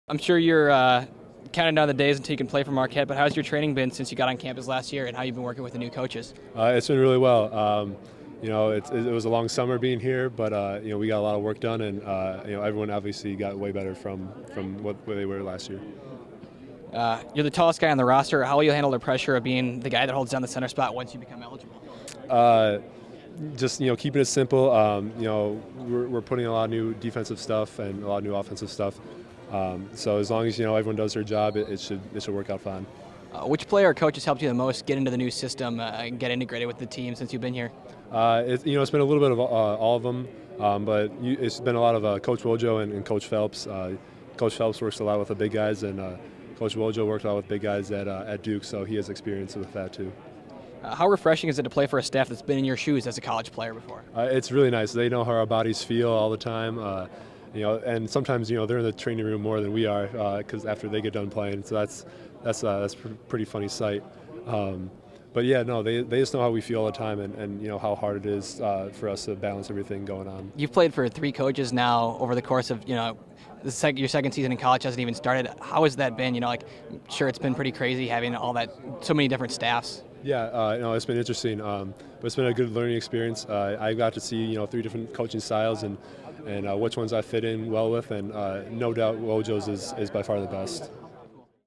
MUBB 1-on-1 with Luke Fischer
At Marquette men's basketball media day MUTV's Grant Becker caught up with sophomore center Luke Fischer. The Germantown, WI native talks about working with the new coaching staff and being the tallest player on the team.